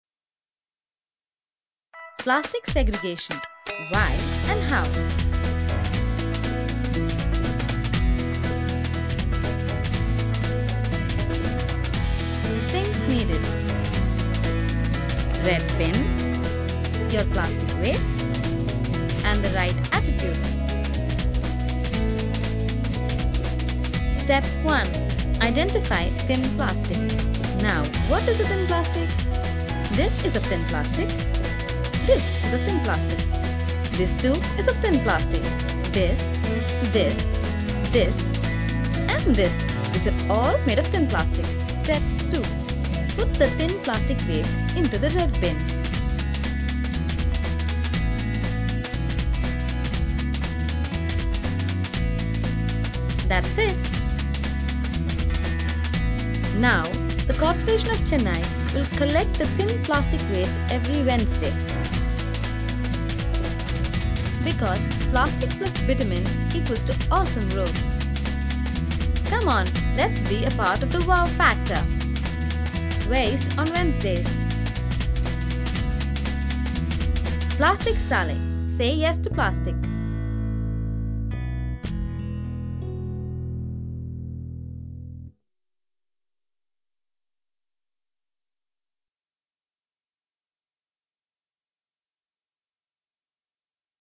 Plastic Salai - the WOW factor
This video talks about the how and why of Segregation of plastic waste for plastic roads.